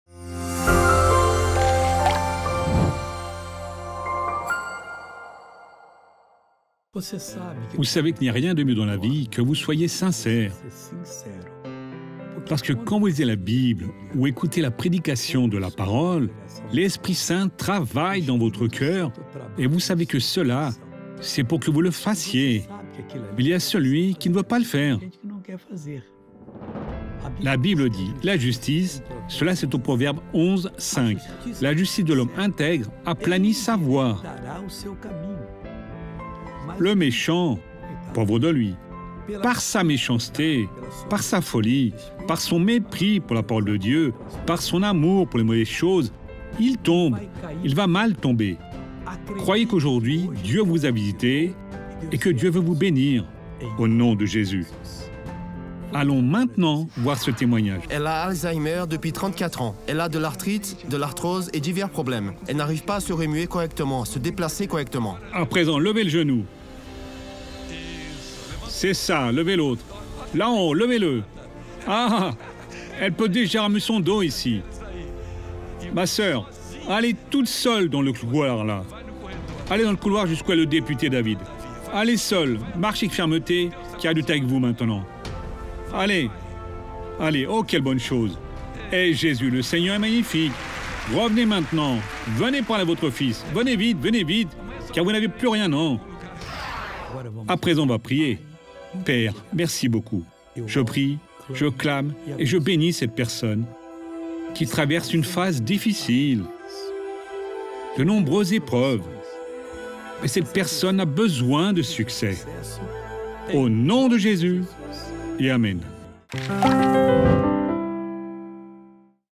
Pourquoi Dieu a-t-Il tant de plaisir avec les gens sincères?
L´impie - qui est celui qui ne garde pas ce que Dieu dit – il vit trébuchant dans la vie. Pour cette raison, vous devez faire attention à l´enseignement de ce bref message afin de ne pas avoir le même destin!